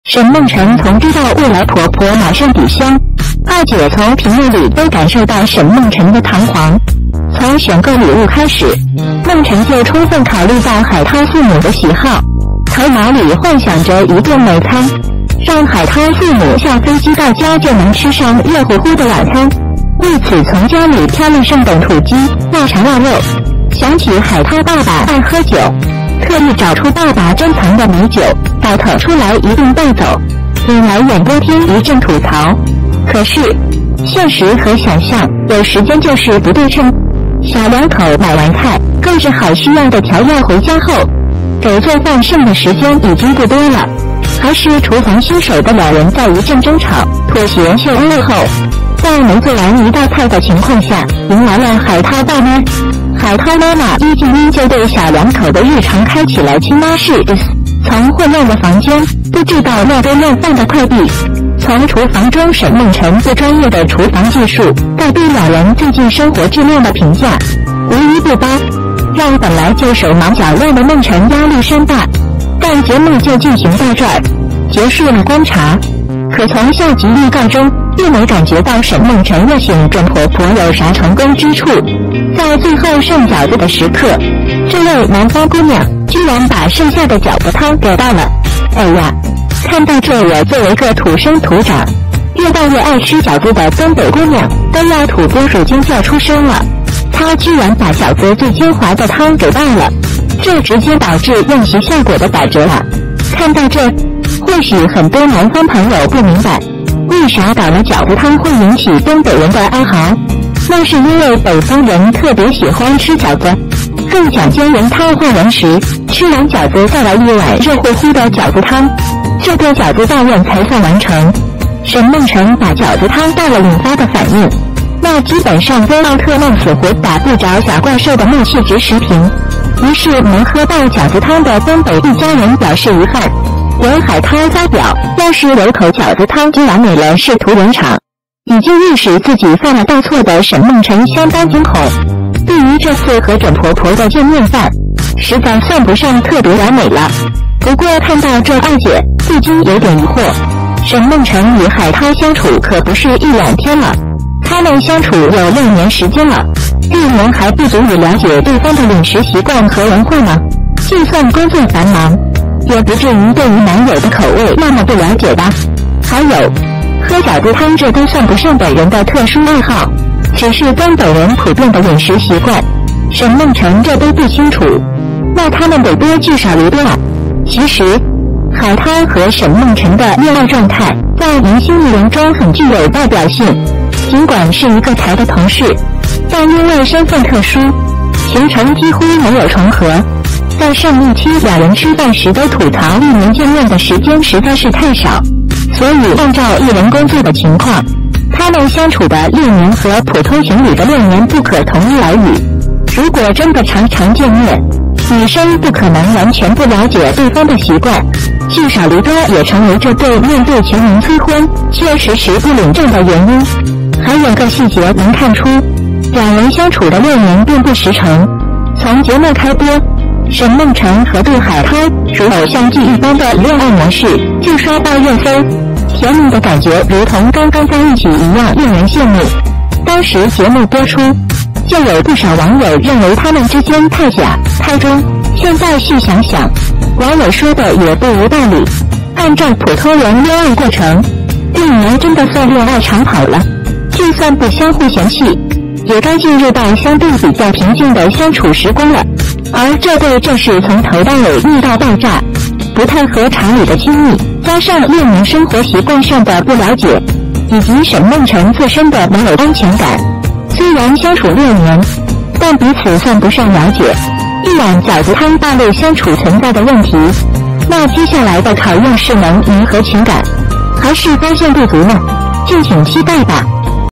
一碗饺子汤暴露梦辰海涛习惯差异大，6年感情没经住饺子汤的考验
沈梦辰从知道未来婆婆马上抵湘，贰姐从屏幕里都感受到沈梦辰的堂皇，从选购礼物开始，梦辰就充分考虑到海涛父母的喜好，头脑里幻想着一顿美餐，让海涛父母下飞机到家就能吃上热乎乎的晚餐，为此从家里挑了上等土鸡、腊肠腊肉，想起海涛爸爸爱喝酒，特意找出爸爸珍藏的美酒倒腾出来一并带走，引来演播厅一阵吐槽。可是，现实和想象有时间就是不对称，小两口买完菜、购置好需要的调料回家后，给做饭剩的时间已经不多了，还是厨房新手的两人在一阵争吵、妥协秀恩爱后，在没做完一道菜的情况下迎来了海涛爸妈，海涛妈妈一进屋就对小两口的日常开启了“亲妈式diss”，从混乱的房间布置到乱堆乱放的快递，从厨房中沈梦辰不专业的厨房技术到对两人最近生活质量的评价，无一不包，让本来就手忙脚乱的梦辰压力山大。但节目就进行到这儿，结束了观察，可从下集预告中并没感觉到沈梦辰宴请准婆婆有啥成功之处，在最后上饺子的时刻，这位南方姑娘居然把剩下的饺子汤给倒了！诶呀，看到这我作为个土生土长，越大越爱吃饺子的东北姑娘都要“土拨鼠惊叫”出声了，她居然把饺子最精华的汤给倒了，这直接导致宴席效果的打折啊。看到这，或许很多南方朋友不明白，为啥倒了饺子汤会引起东北人的哀嚎，那是因为北方人特别喜欢吃饺子，更讲究“原汤化原食”，吃完饺子再来一碗热乎乎的饺子汤，这顿饺子大宴才算完成，沈梦辰把饺子汤倒了引发的反应，那基本上跟奥特曼死活打不着小怪兽的怒气值持平。于是没喝到饺子汤的东北一家人表示遗憾，由海涛发表“要是有口饺子汤就完美了”试图圆场，已经意识自己犯了大错的沈梦辰相当惊恐，对于这次和准婆婆的见面饭，实在算不上特别完美了。不过看到这贰姐不禁有点疑惑，沈梦辰与海涛相处可不是一两天了，他们相处有6年时间了，6年还不足以了解对方的饮食习惯和文化吗？就算工作繁忙，也不至于对于男友的口味那么不了解吧，还有，喝饺子汤这都算不上本人的特殊爱好，只是东北人普遍的饮食习惯，沈梦辰这都不清楚，那他们得多“聚少离多”啊！其实，海涛和沈梦辰的恋爱状态在明星艺人中很具有代表性，尽管是一个台的同事，但因为身份特殊，行程几乎没有重合，在上一期两人吃饭时都吐槽一年见面的时间实在是太少，所以按照艺人工作的情况，他们相处的6年和普通情侣的6年不可同日而语，如果真的常常见面，女生不可能完全不了解对方的习惯，聚少离多也成为这对面对全民催婚却迟迟不领证的原因。还有个细节能看出，两人相处的6年并不实诚。从节目开播，沈梦辰和杜海涛如偶像剧一般的恋爱模式就刷爆热搜，甜蜜的感觉如同刚刚在一起一样令人羡慕。当时节目播出，就有不少网友认为他们之间“太假、太装”，现在细想想，网友说的也不无道理。按照普通人恋爱过程，6年真的算恋爱长跑了，就算不相互嫌弃，也该进入到相对比较平静的相处时光了，而这对这是从头到尾腻到爆炸，不太和常理的亲密加上6年生活习惯上的不了解，以及沈梦辰自身的没有安全感，虽然相处6年，但彼此算不上了解，一碗饺子汤暴露相处存在的问题，那接下来的考验是能弥合情感，还是发现不足呢？敬请期待吧。